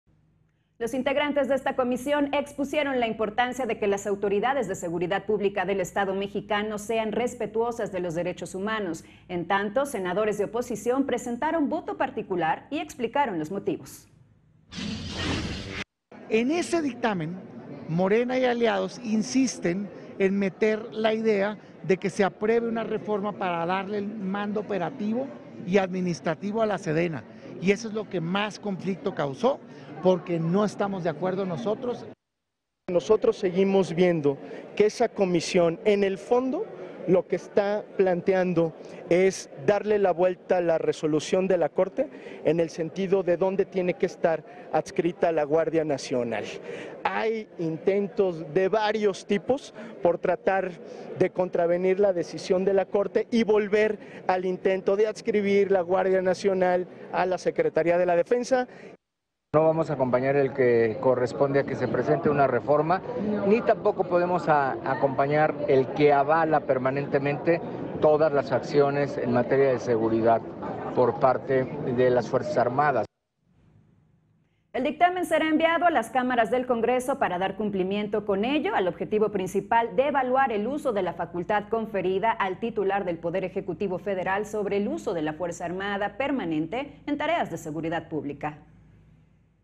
Resaltan importancia de garantizar derechos humanos
Los integrantes de esta comisión expusieron la importancia de que las autoridades de seguridad pública del Estado mexicano sean respetuosas de los derechos humanos. En tanto senadores de oposición presentaron voto particular y explicaron los motivos.